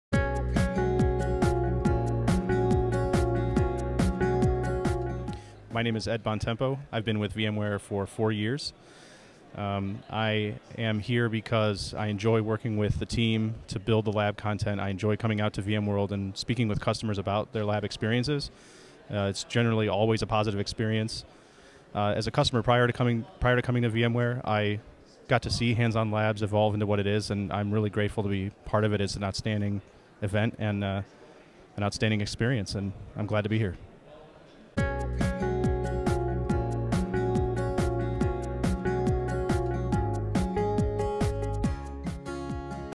Hear From The Experts At VMworld: Ed Bontempo
At VMworld 2018, we captured footage of the Hands-on Labs team. Meet Ed Bontempo and hear about his experience. Test-drive VMware Products